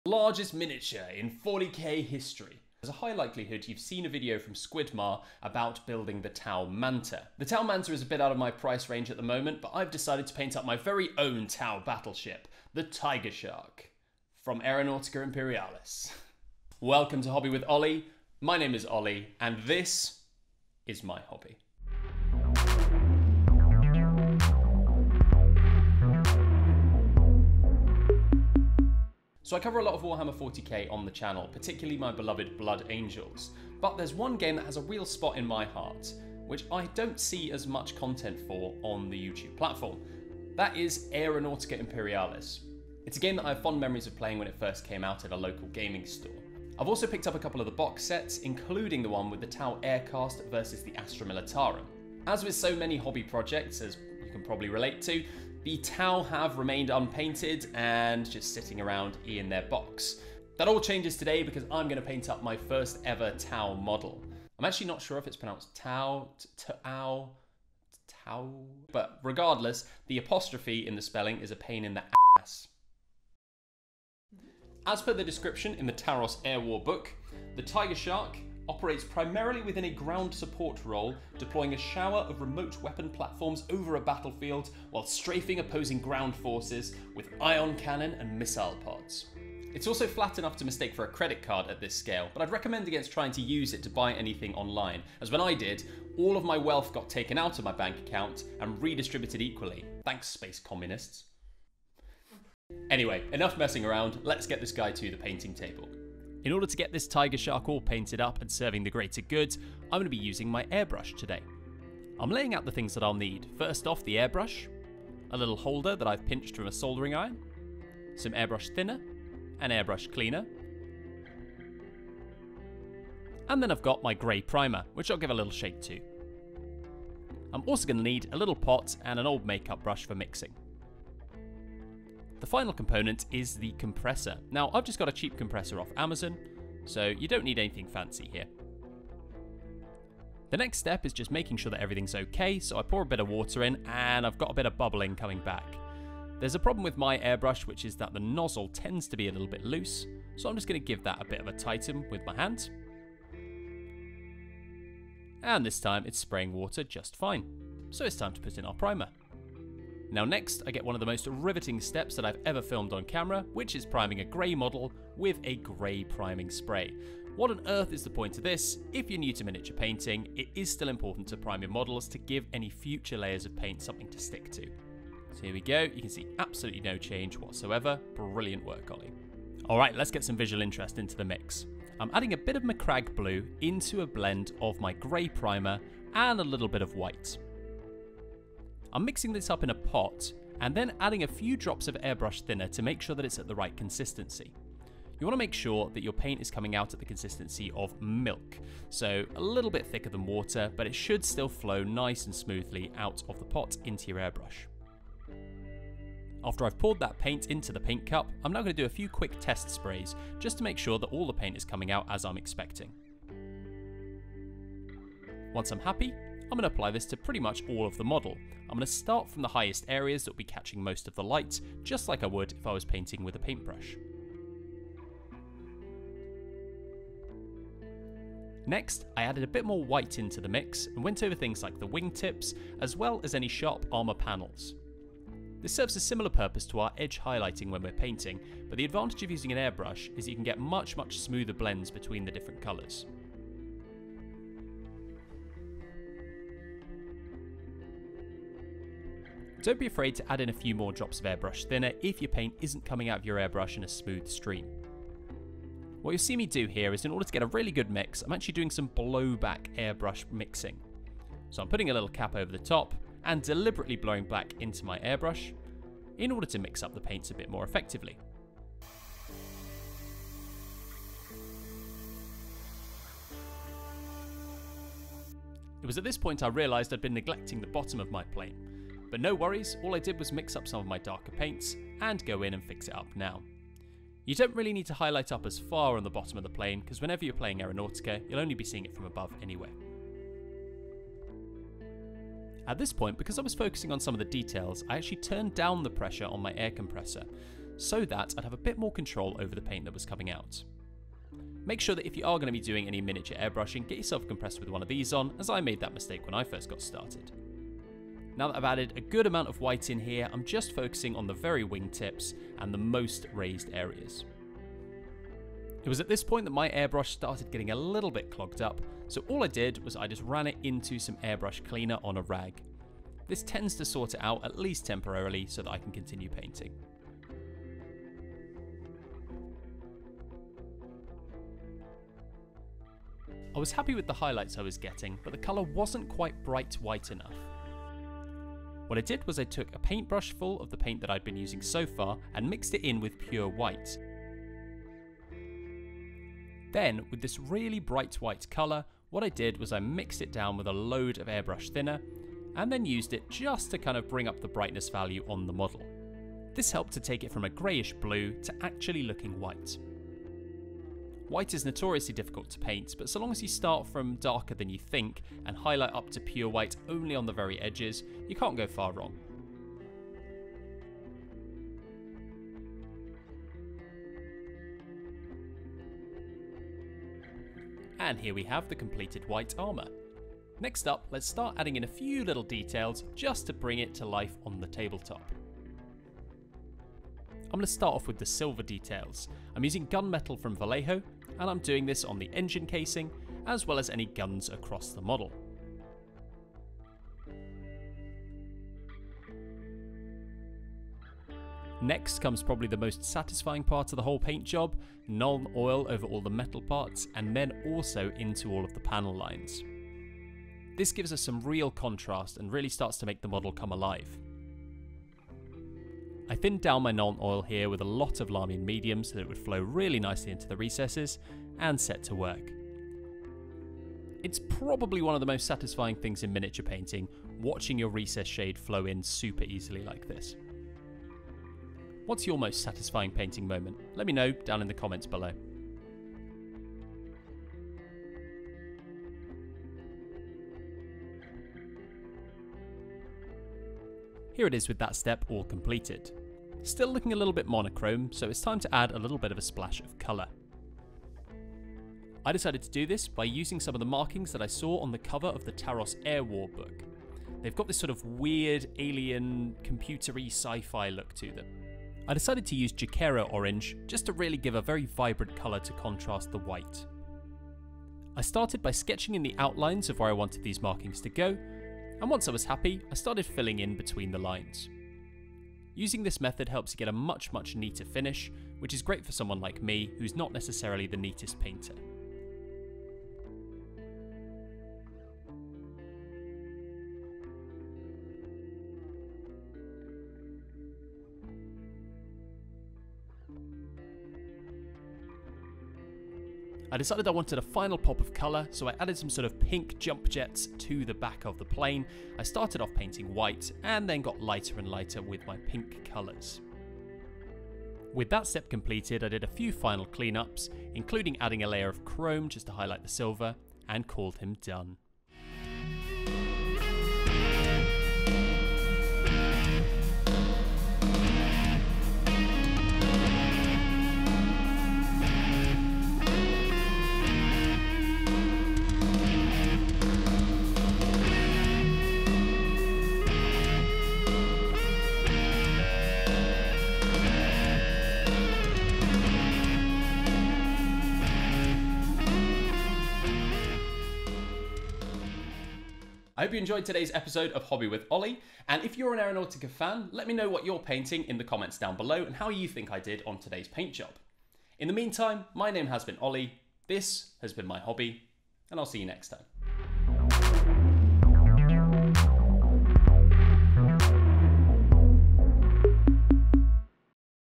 My First ever T'au model!- Aeronautica Imperialis T'au Tiger Shark Painting Guide
Taking a break from standard 40k to talk about Aeronautica Imperialis! One of my favourite games brought back by Games Workshop in recent years. Also painting up my first ever T'au model!